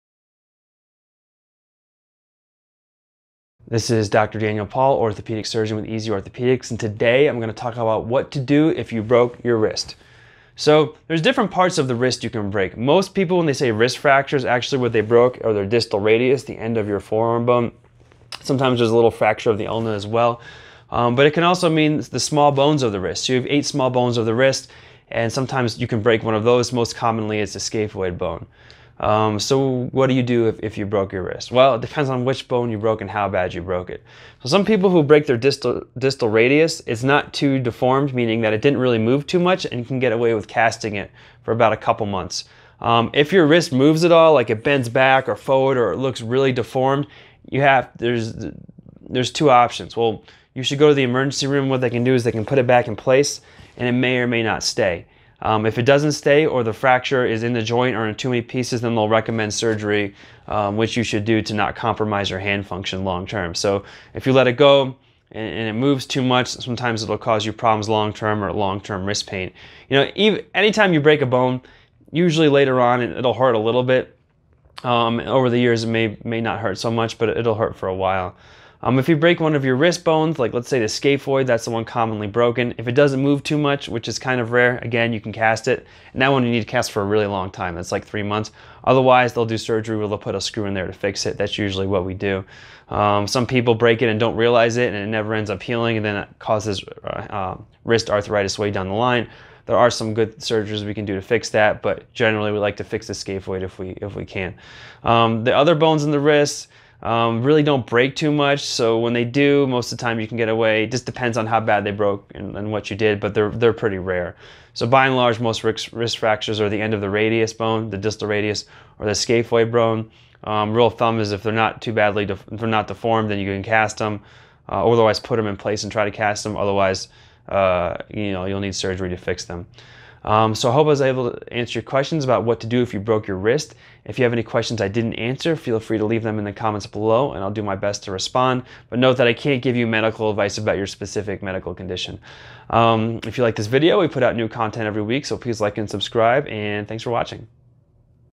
What to do if you broke your wrist
The wrist has several bones, but most commonly people brake their distal radius or scaphoid. Depending on how badly your broke your wrist and how displaced the bones are you can heal with a cast. However, a badly displaced or multiple bone brake may require surgery.